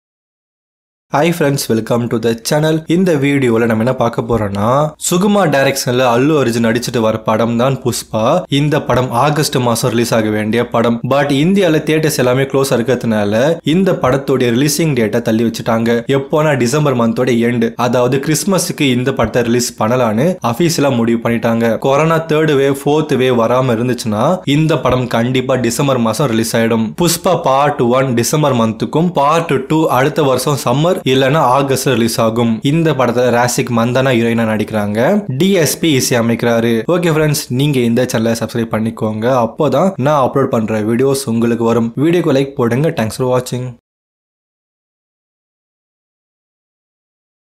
Pushpa Release Date Changed | Allu Arjun | Rashmika | Sukumar
In This Video I Shared The Information About.

1. Allu Arjun Upcoming Movie Pushpa Release Date Is Changed To Christmas.